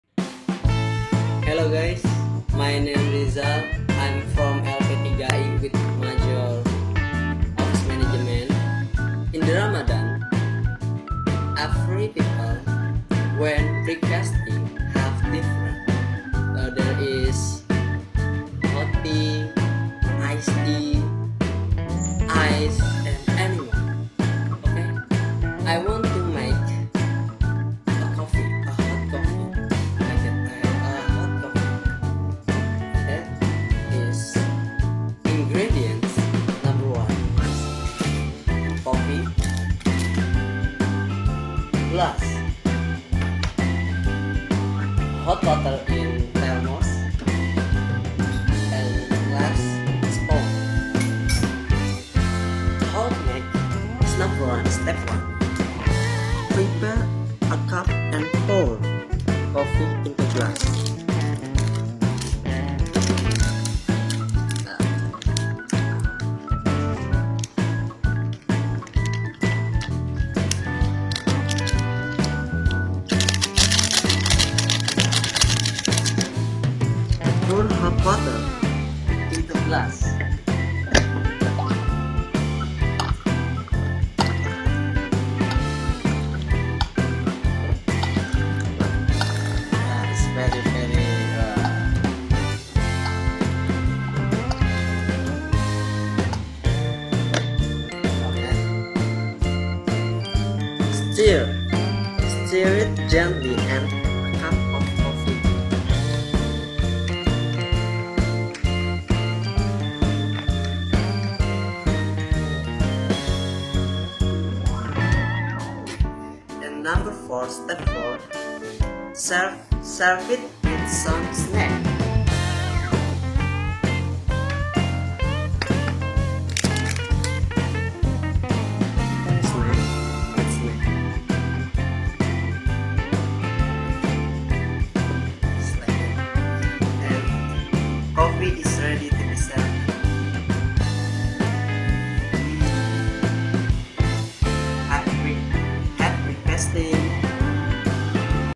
Hot coffee or spicy coffee..by Rizal Septian
It's very interesting and rare experiment from Rizal Septian... Spicy Coffee??